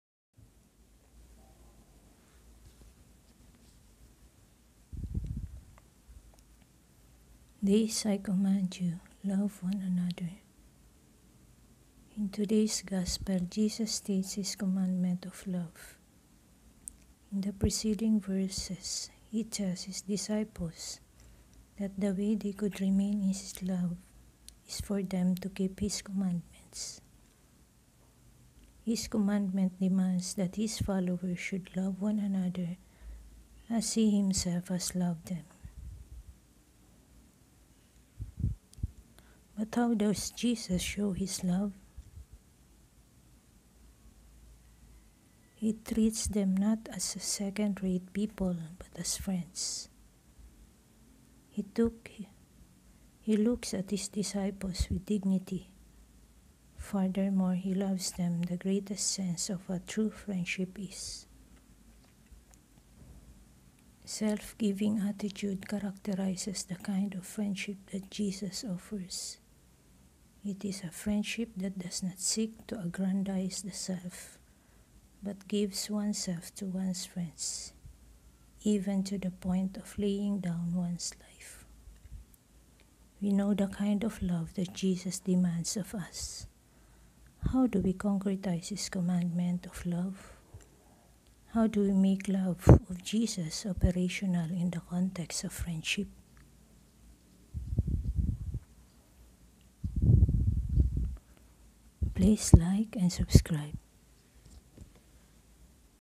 This I command you: love one another.
His commandment demands that his followers should love one another as he himself has loved them.